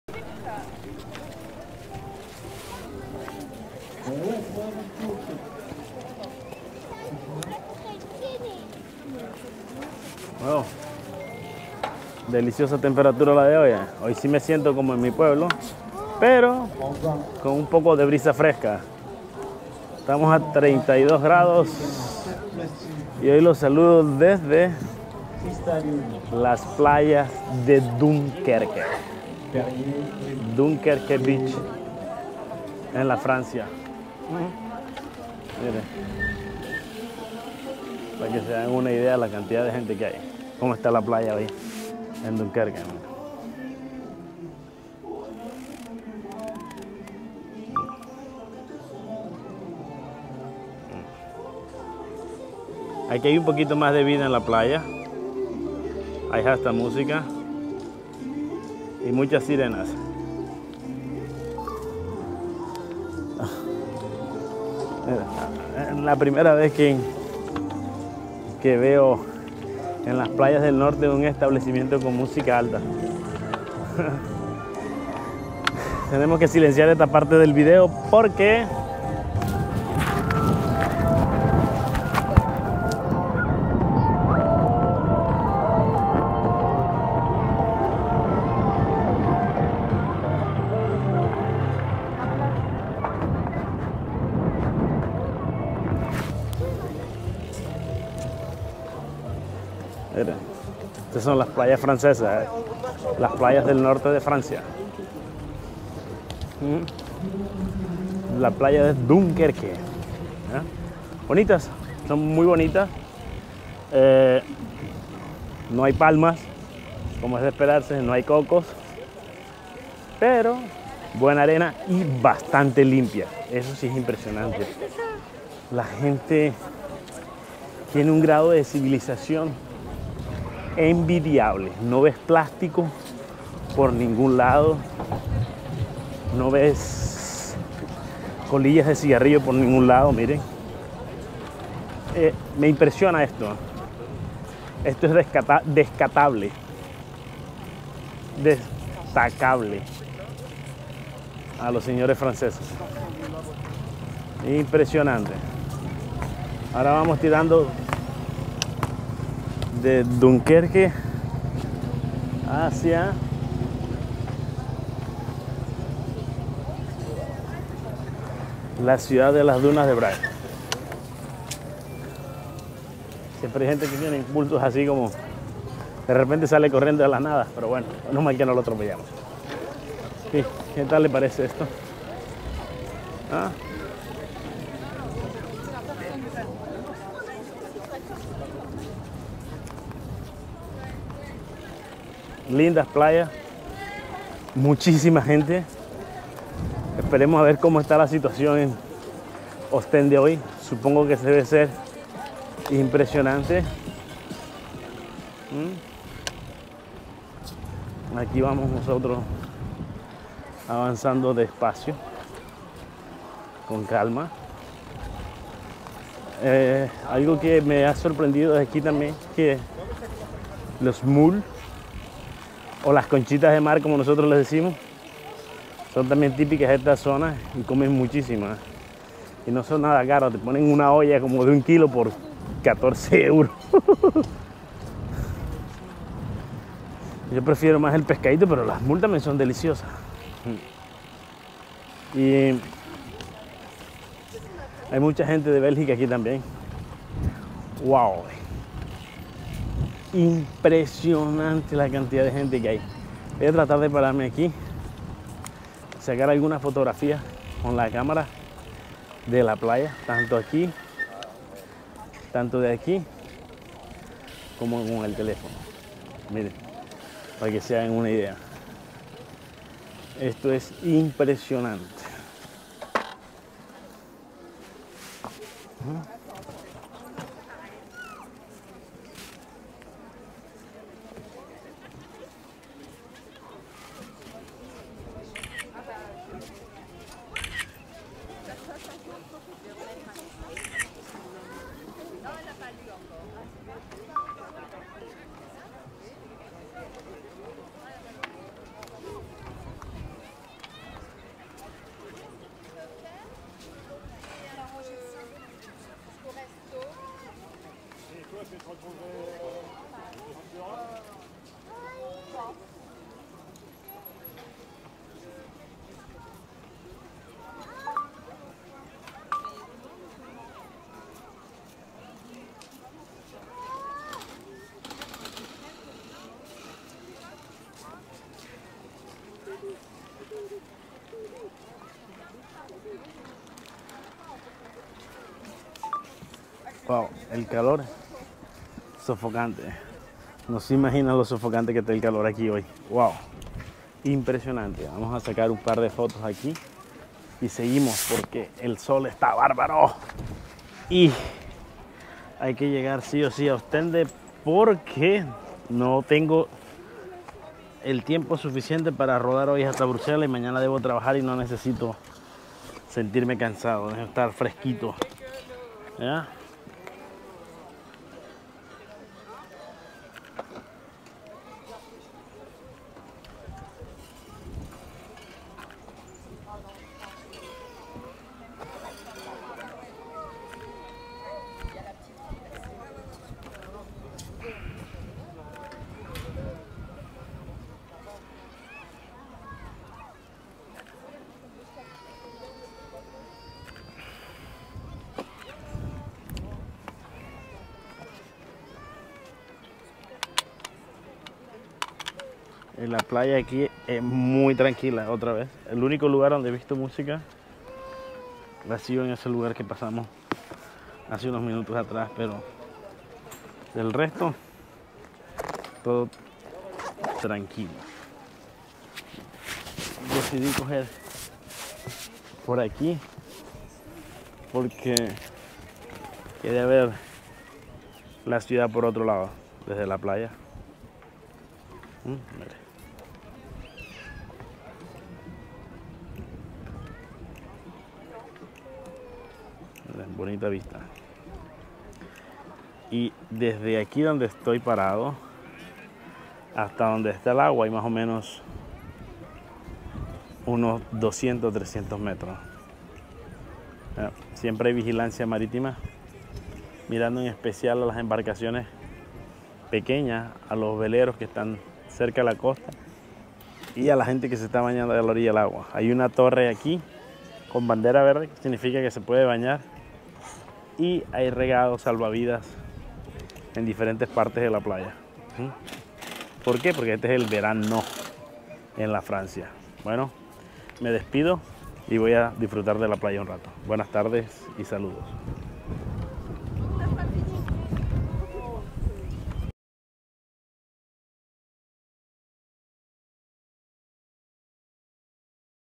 DUNQUERQUE PLAGE, DUNKERQUE BEACH en FRANCIA, VIAJE EN BICICLETA.
Descubramos juntos la magia y belleza de las playas de la ciudad de Dunkerque en el norte de Francia, ciudad histórica con unas encantadoras playas.